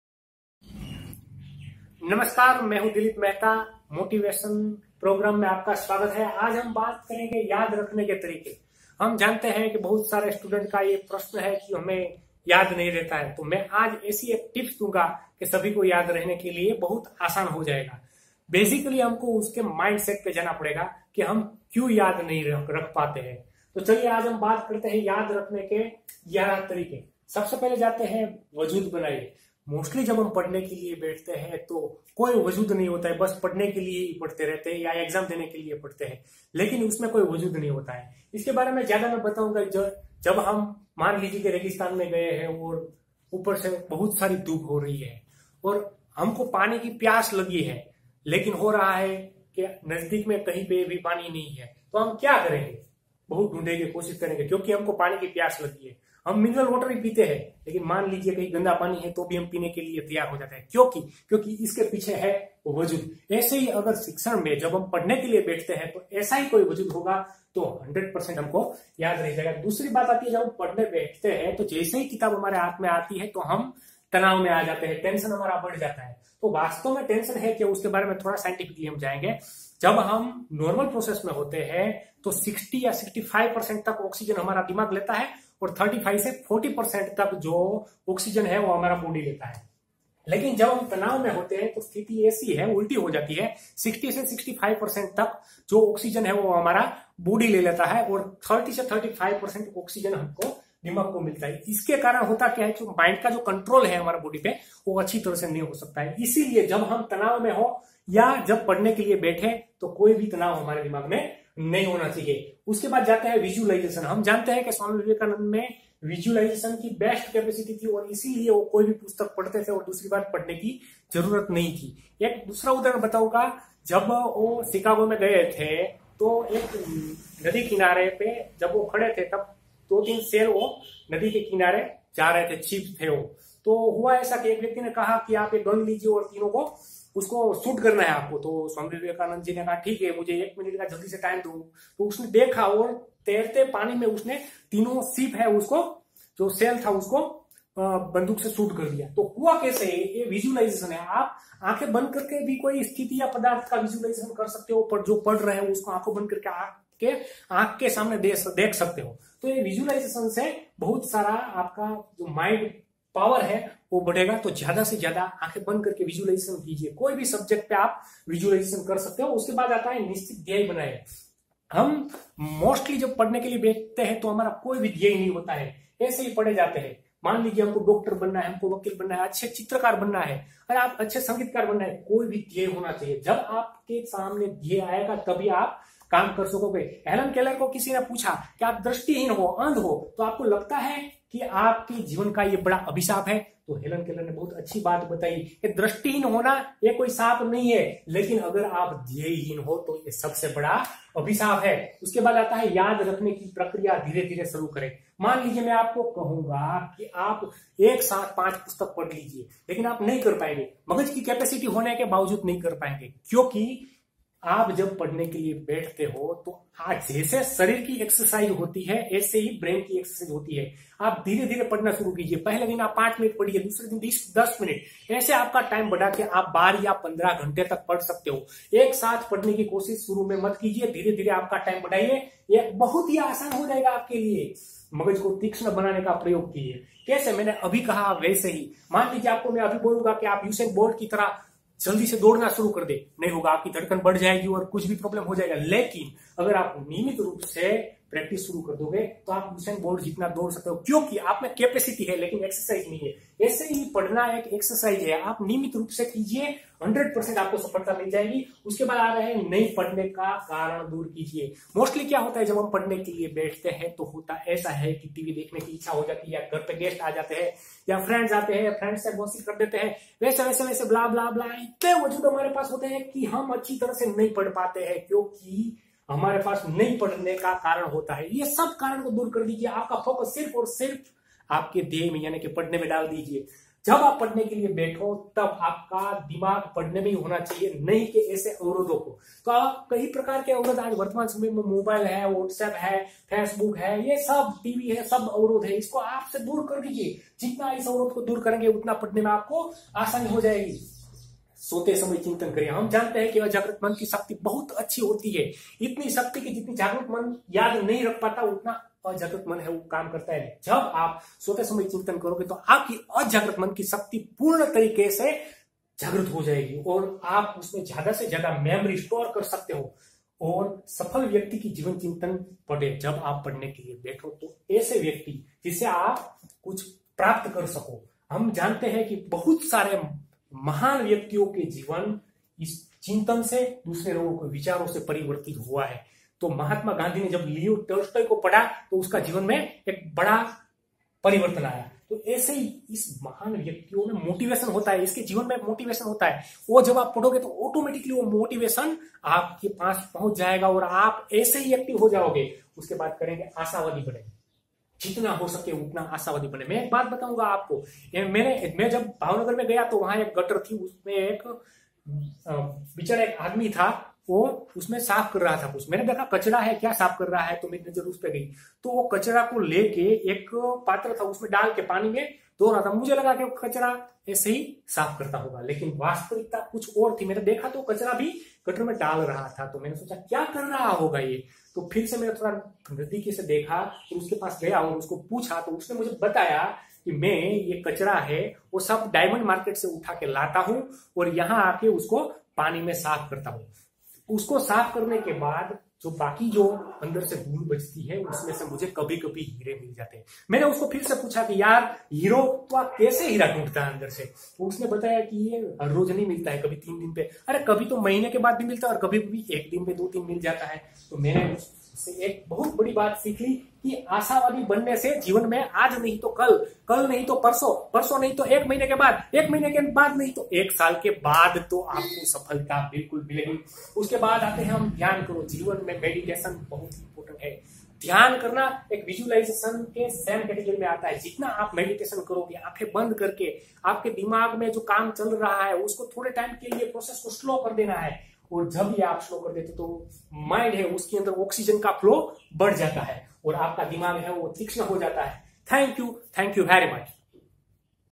How To Remember Things Easily - Long Term Yaad Kaise rakhe
How To Remember Things Easily   Long Term Yaad Kaise rakhe by D K Mehta
याद कैसे रखें
Aasani se yad rakhane ke 11 tarike
aasani se yad rakhe